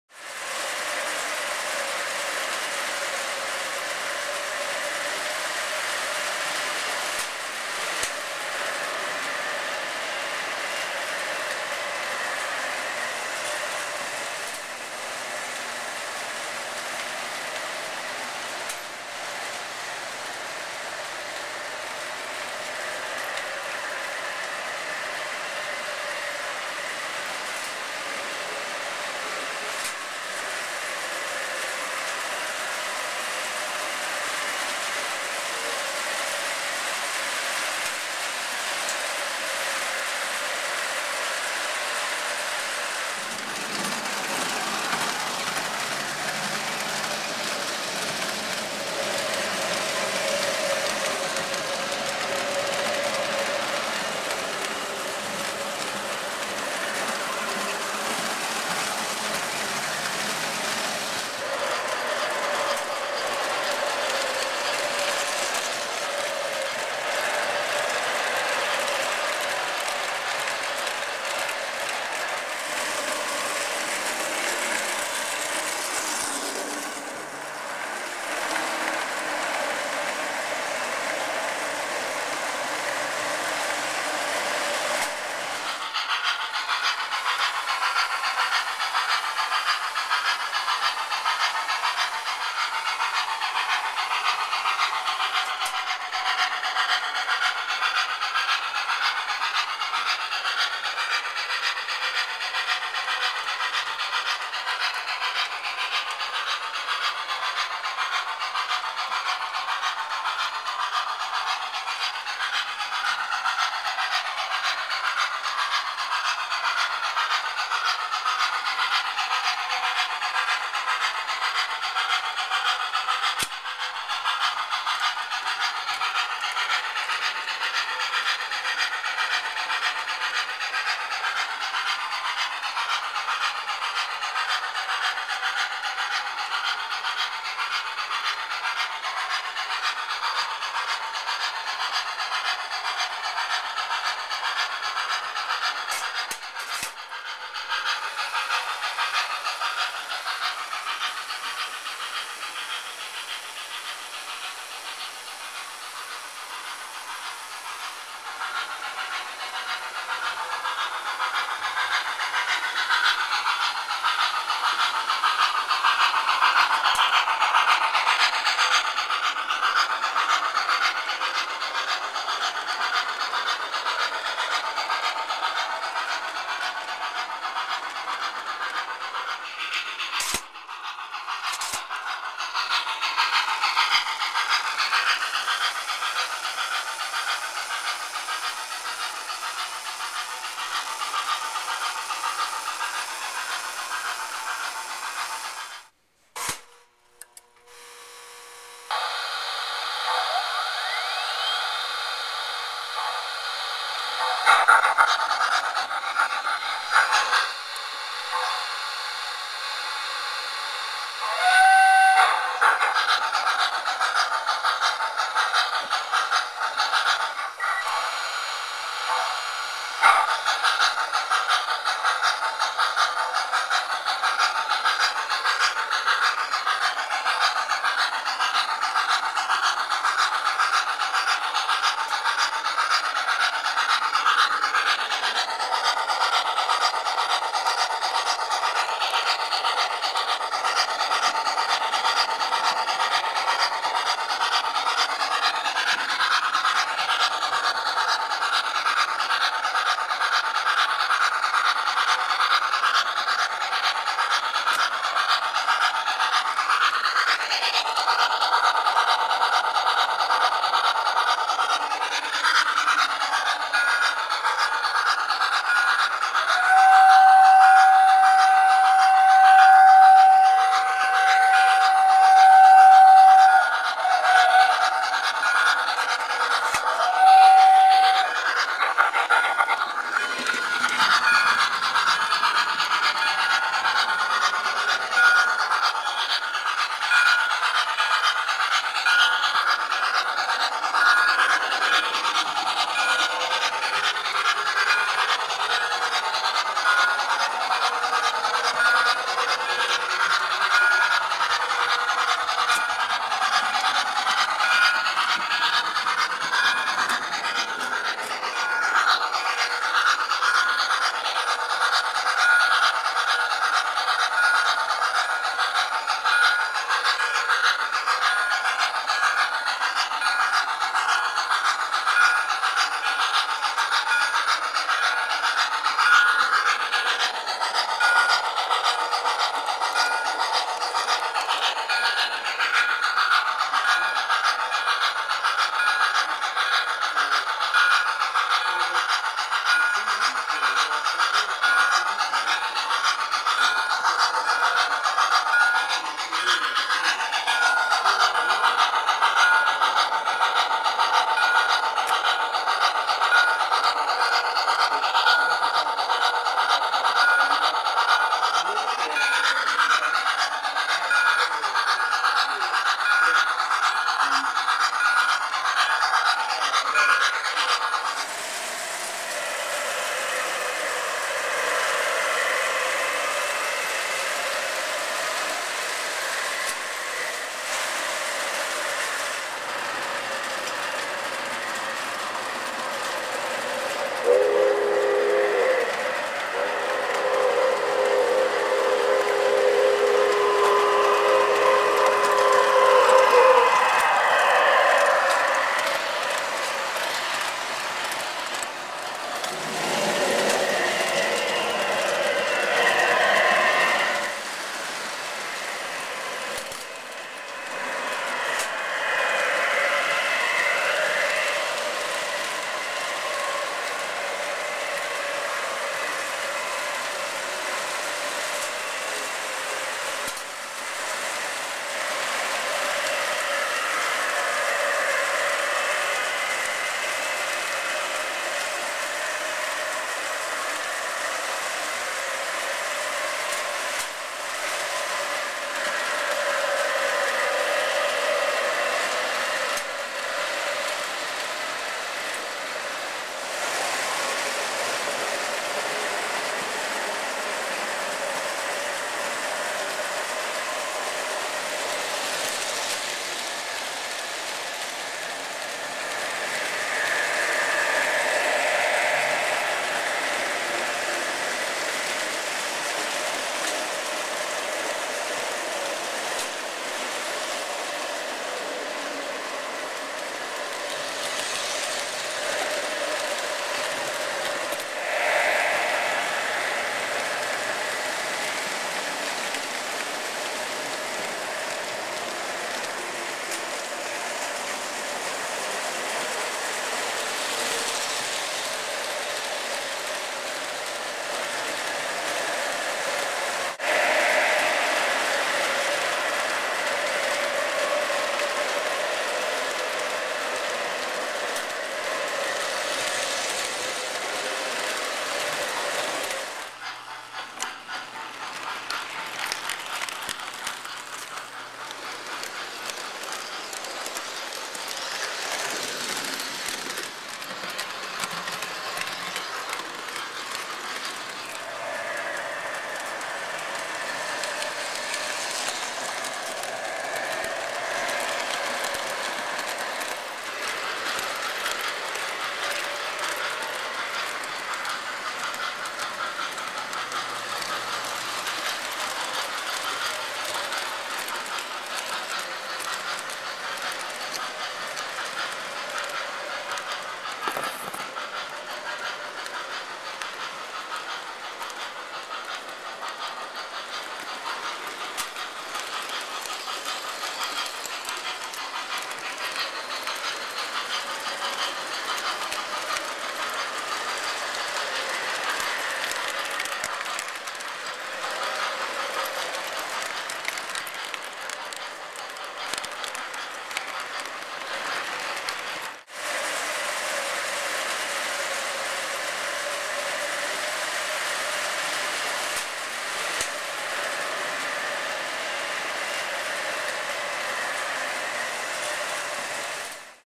TRAINS, TRAINS & MORE TRAINS
This O-Guage layout is over 20 years old and is full of post-war, modern-era, and trains and accessories.